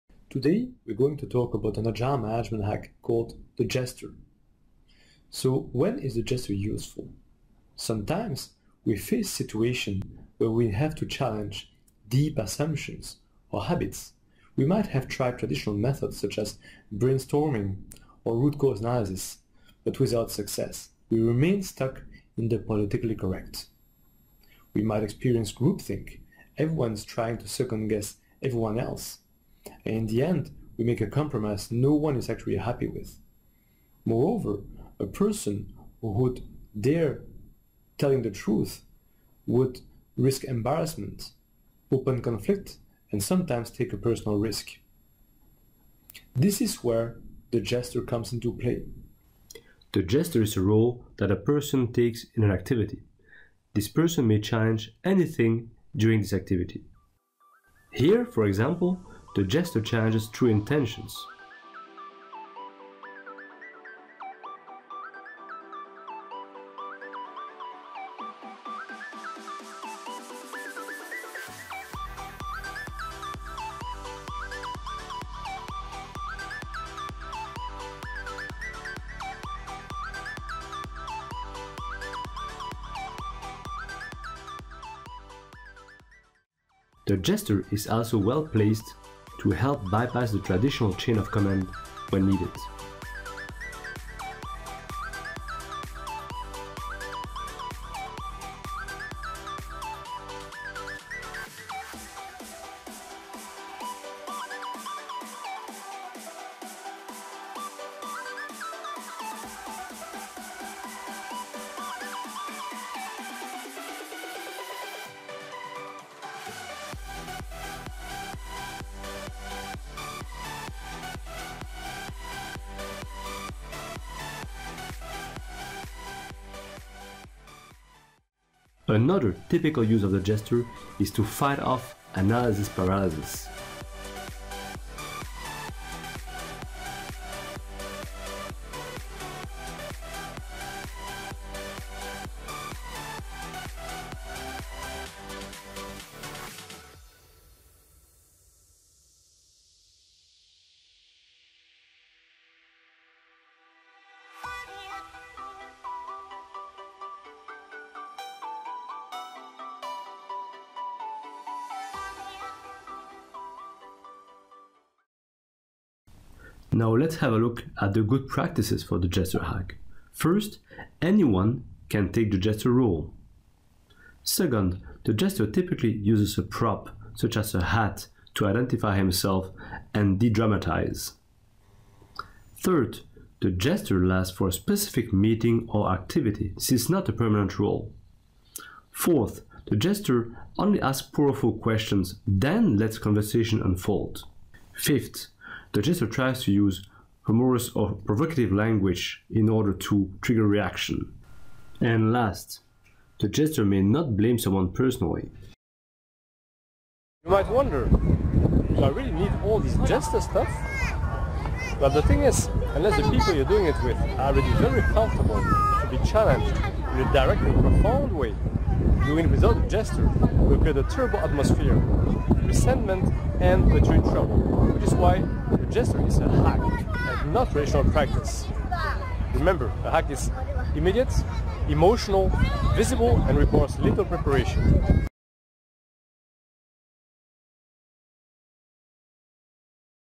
The Jester or How to shake up your politically correct meetings
When your team is stuck in consensus or "groupthink" preventing creative thinking, then the Jester agile management hack will help you challenge habits and political correctness in order to get you unstuck. Plus, it's funny. You need only one thing to try it: courage.
The Jester is inspired from the middle-age jester who, besides entertaining his audience, also helped unveil the inconvenient truth and offer innovative perspective with non-threatening humor and impudence.

for free and original content about leadership agility